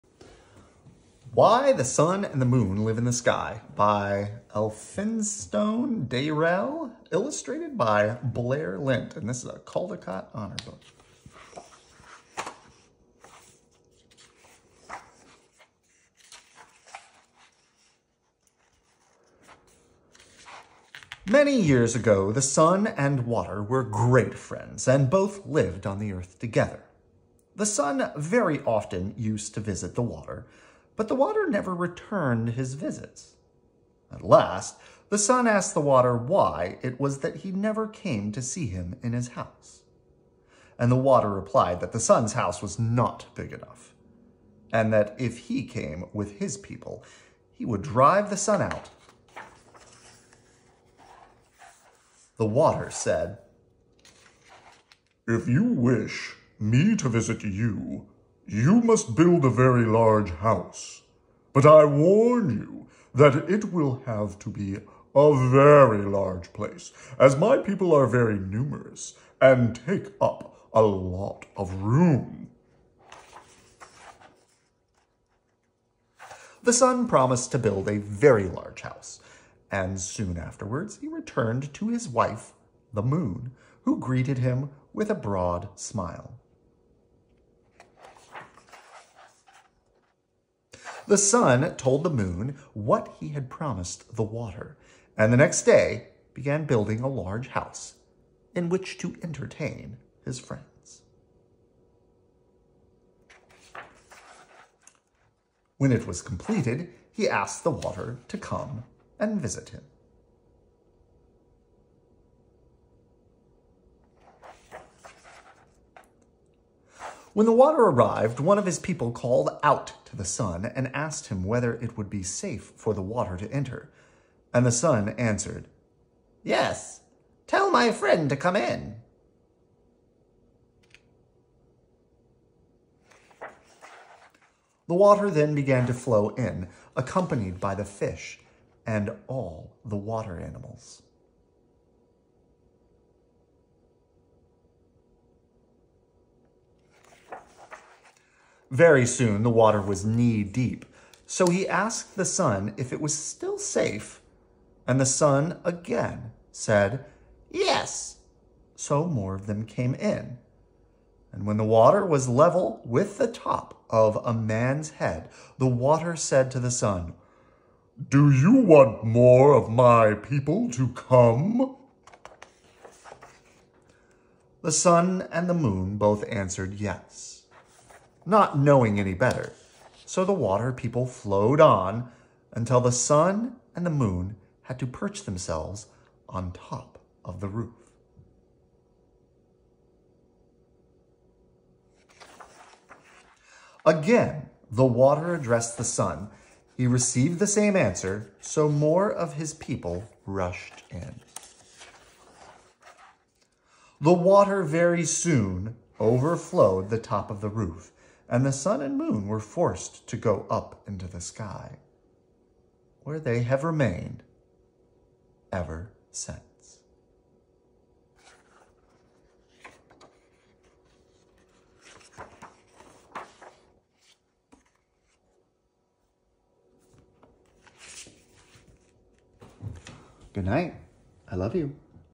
Ash reads Why the Sun and the Moon Live in the Sky by Elphinstone Dayrell illustrated by Blair Lent
Caldecott award
Bedtime stories
Story
Children’s book
Children
Kids
Kid
Read along
Read aloud